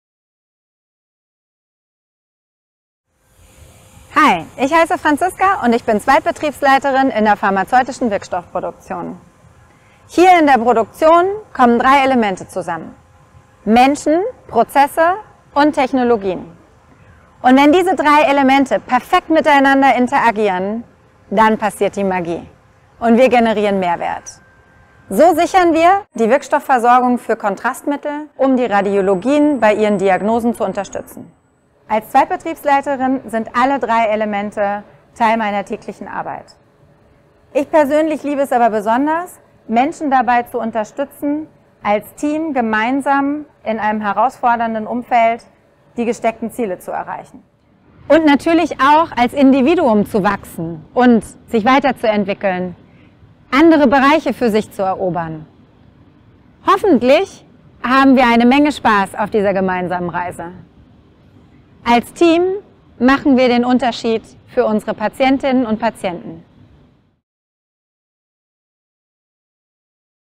Franziska Knauf über ihren Job als Zweitbetriebsleiterin am Bayer-Standort Bergkamen
Die Aufgabe, einen Betrieb zur Produktion pharmazeutischer Wirkstoffe mit zu leiten, ist verantwortungsvoll, abwechslungsreich und erfüllend. Franziska Knauf vom Bayer-Standort Bergkamen erläutert, was sie daran besonders fasziniert.